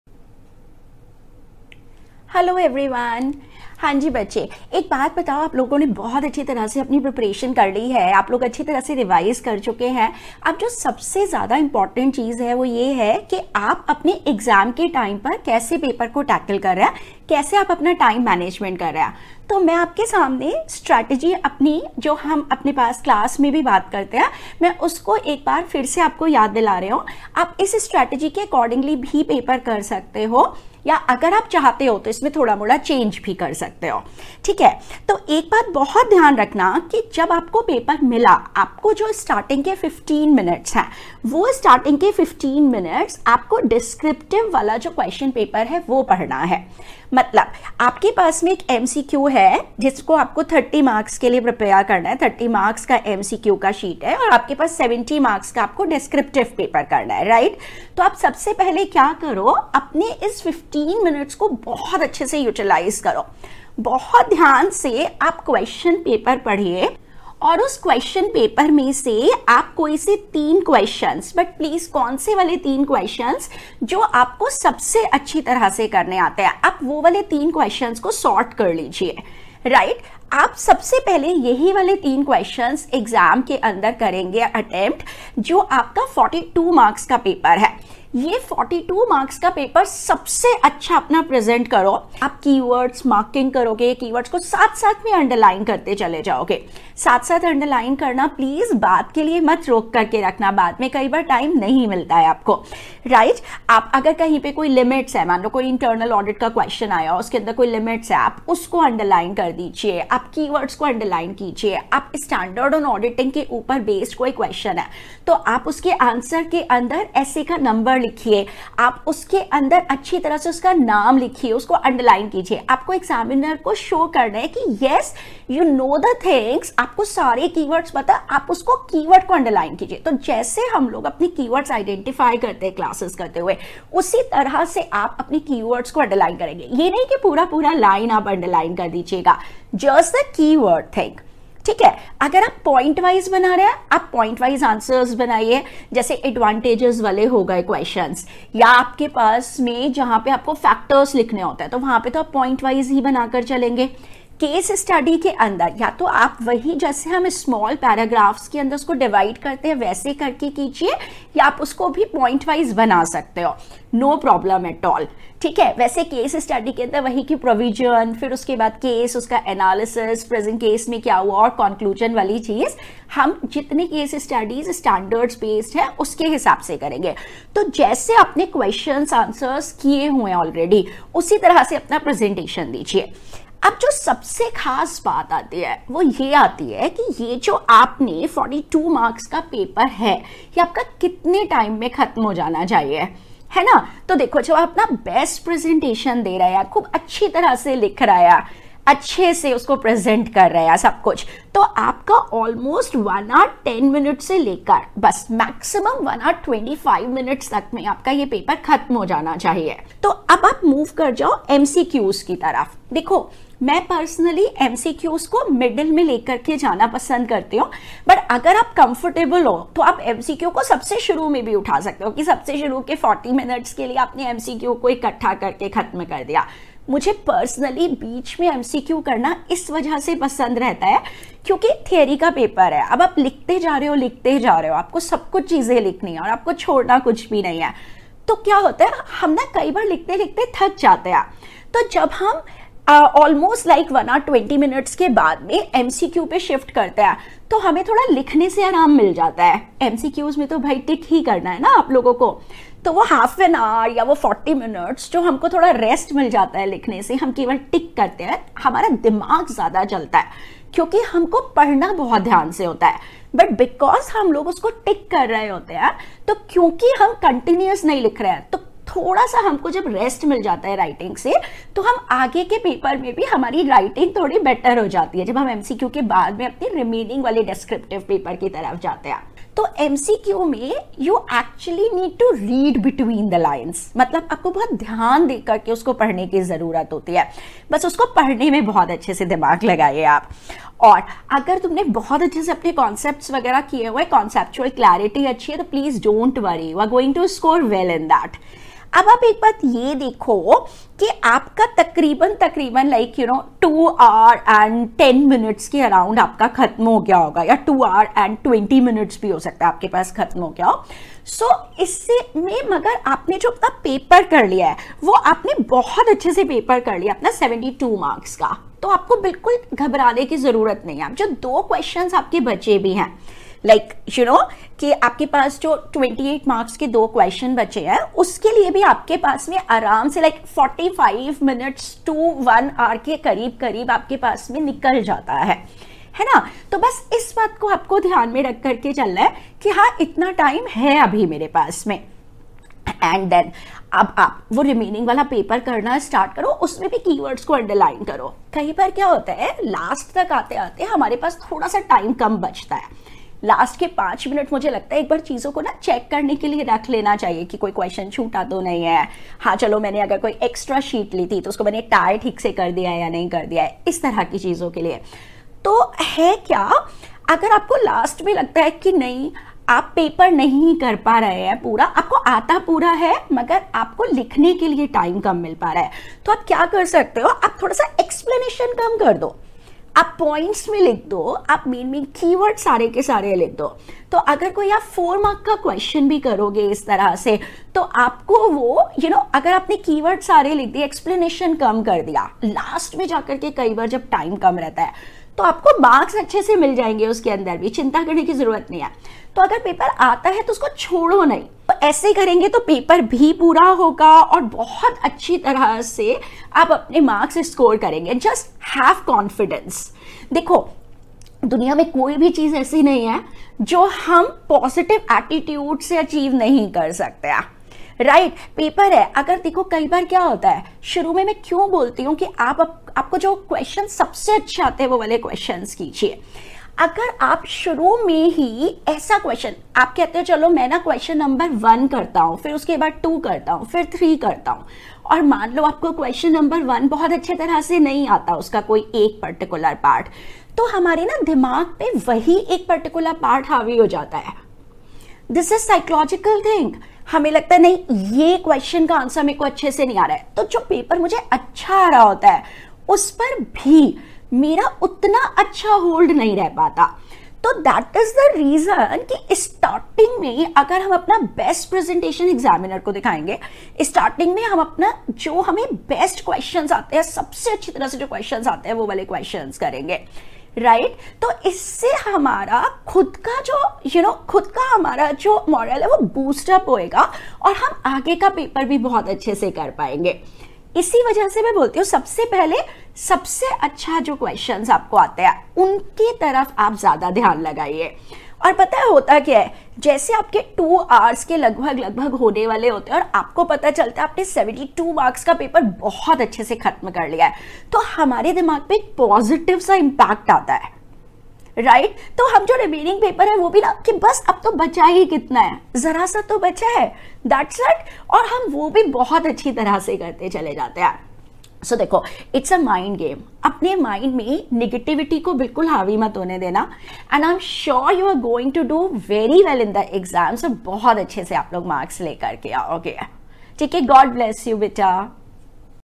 Score Big in CA Inter Auditing & Ethics 💯📚 | Proven Exam Strategy! By CA Surbhi Bansal
🎯 Struggling with how to approach the CA Inter Auditing & Ethics paper? Worry no more! In this video, I'll walk you through the best strategy to attempt the paper, focusing on time management, answering techniques, and key areas to score maximum marks.

📖 Whether you're aiming to master auditing standards or tackle the Ethics section effectively, this step-by-step guide will help you optimize your approach and boost your confidence.

🔑 What You'll Learn:

How to structure your answers for maximum clarity.
Time management tips to finish the paper on time.
Strategies to tackle both theory and practical questions.
✅ Perfect for all CA Inter students looking to refine their exam technique and boost their scores in Auditing & Ethics.